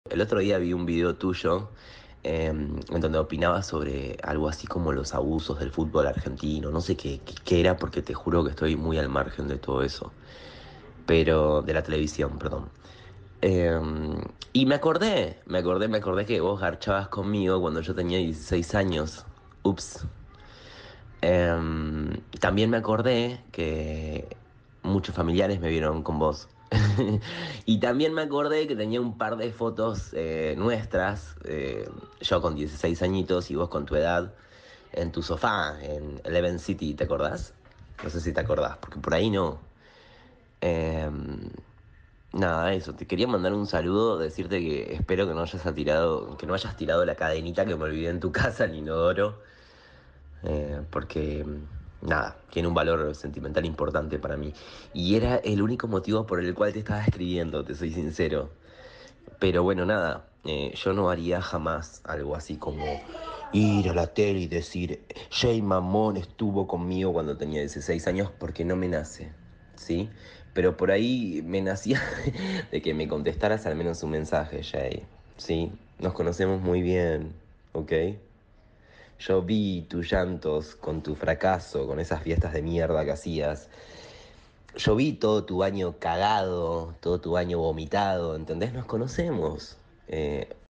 🔥 EL MENSAJE DE VOZ DE LUCAS BENVENUTO A JEY MAMMÓN
-Se difundió este martes un mensaje de voz que Lucas Benvenuto le envió a Jey Mammón tiempo antes de la denuncia que el joven le realizó por presunto abuso sexual. El audio es uno de los materiales que Mammon guarda como prueba frente a las acusaciones y al que se habia referido en las entrevistas televisivas que brindó para afirmar su inocencia.